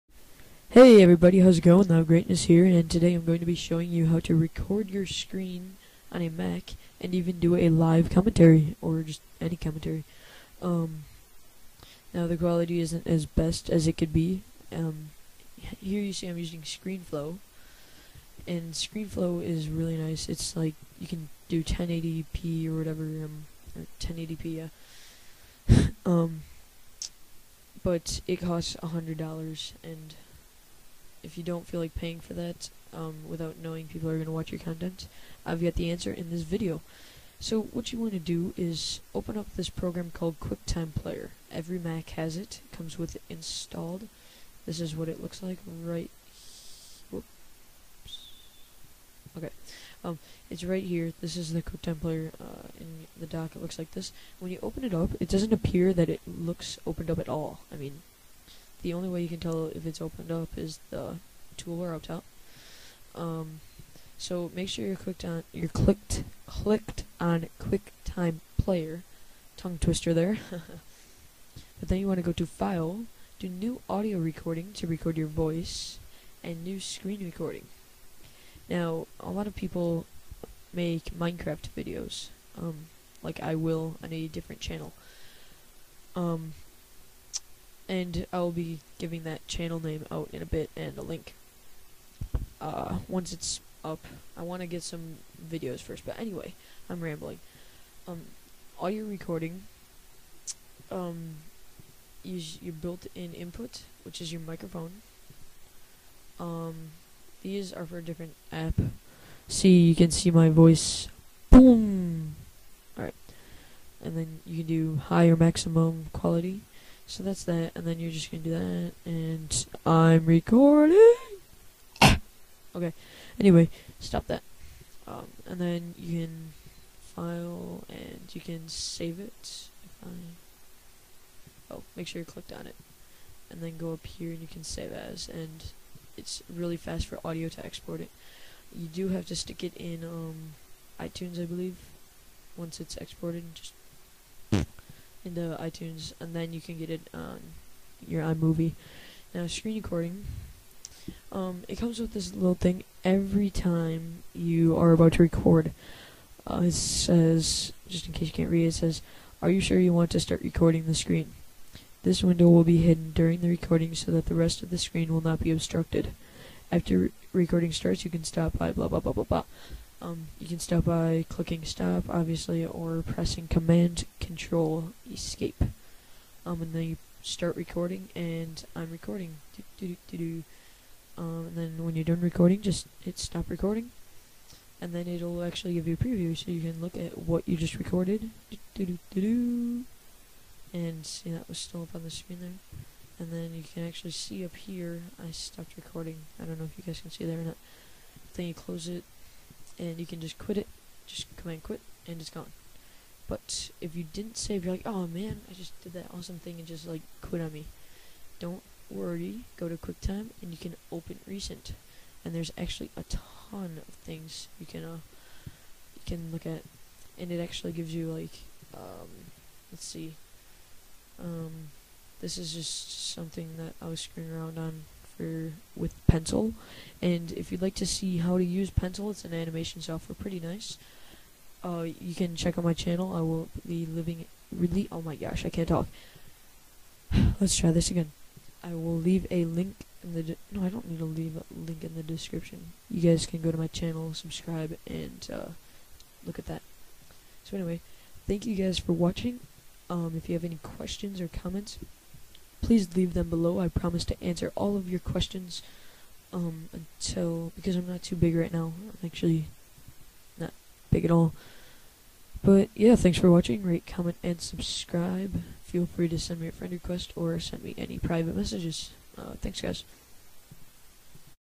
How to record screen on a Mac Free No Download - Live Commentaries!
In this video I show you how to record your screen on a Mac with a completely download free, and cost free program, QuickTime. QuickTime can be used to record screenplay and a mic at the same time. This is great for live commentaries.